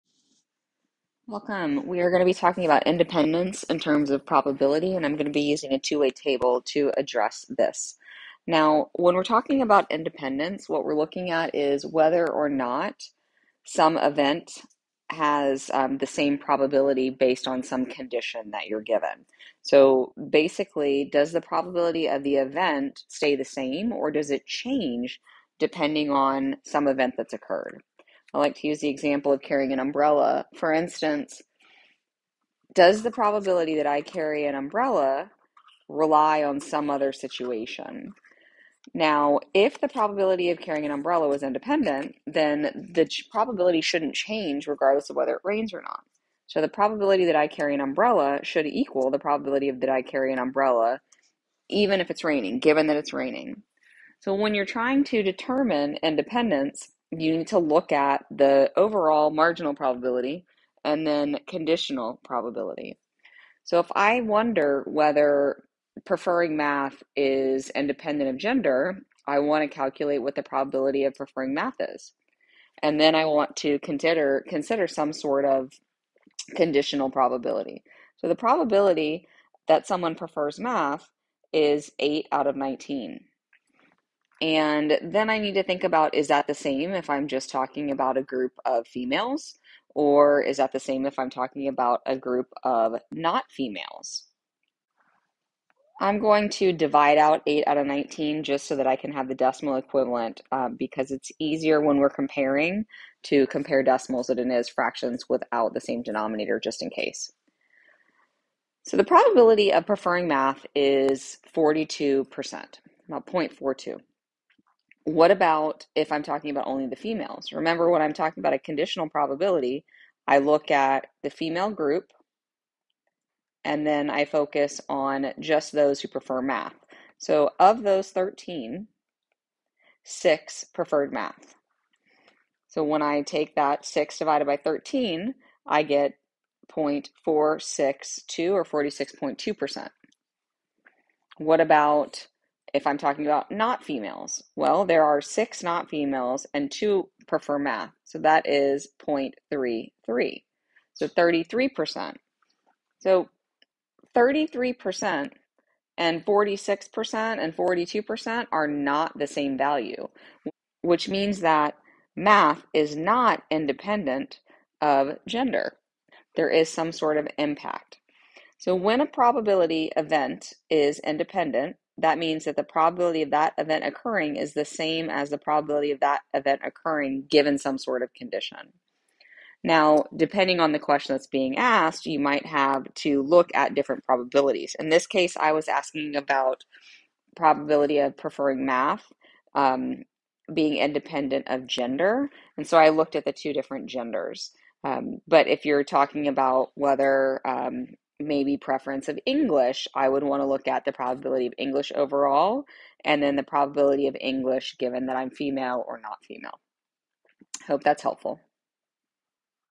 Determining Independence for Probability Using a Two Way Table
Using a two-way table to determine if two events are independent.